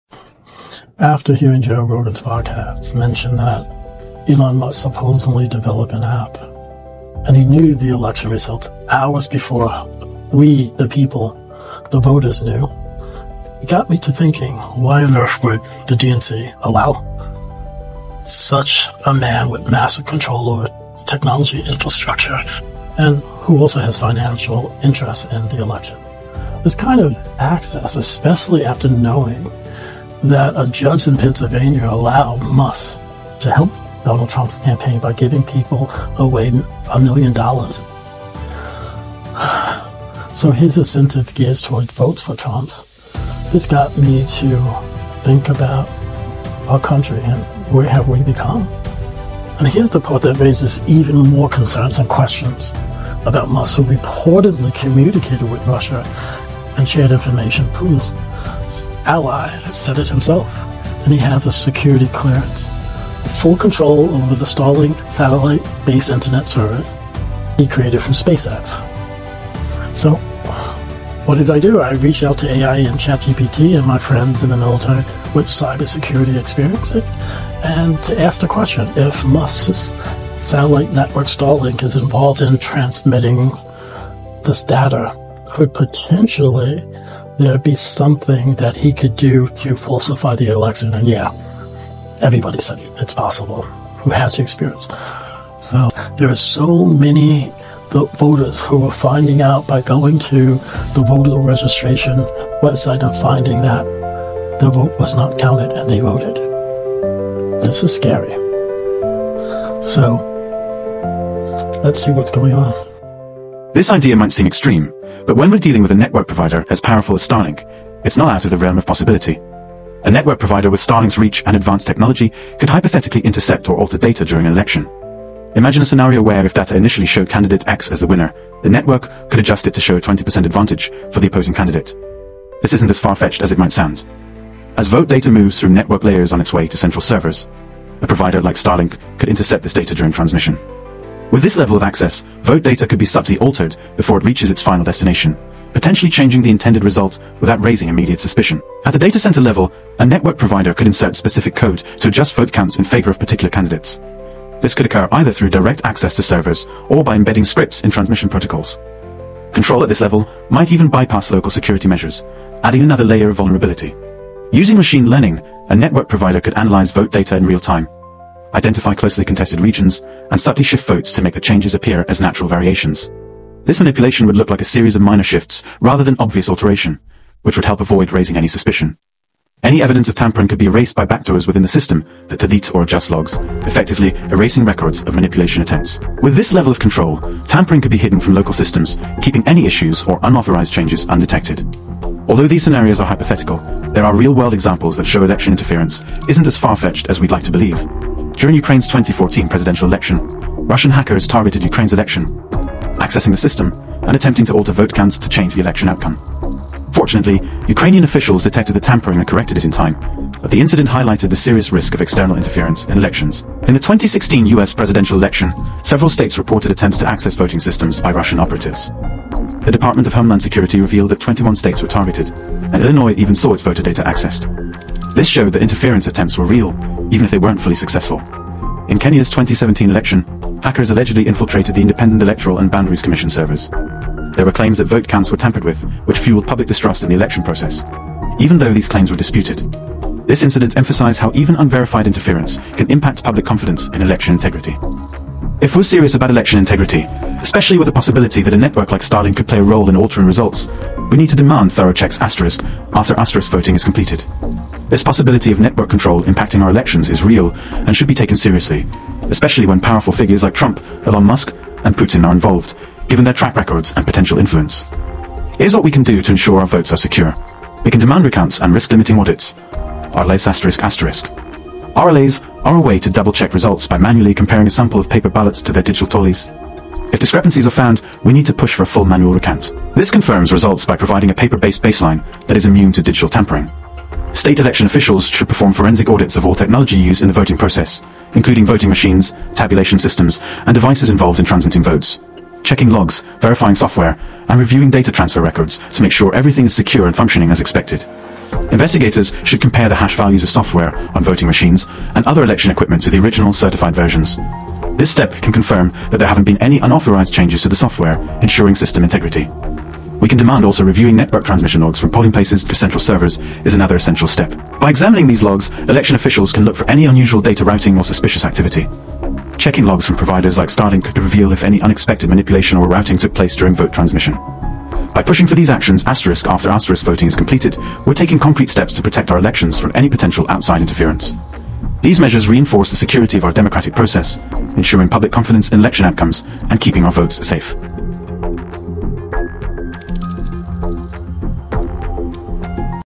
Elon Musk, Putin, and Statlink Interferene potential
starlink election putin Iasked ChatGPT and AI if it’s possible that Starlink by SpaceX could interfere in this election. Elon Musk could write a code in the software to substitute a certain percentage of X (Harris) by 20% with Trump. listen to the video to find out.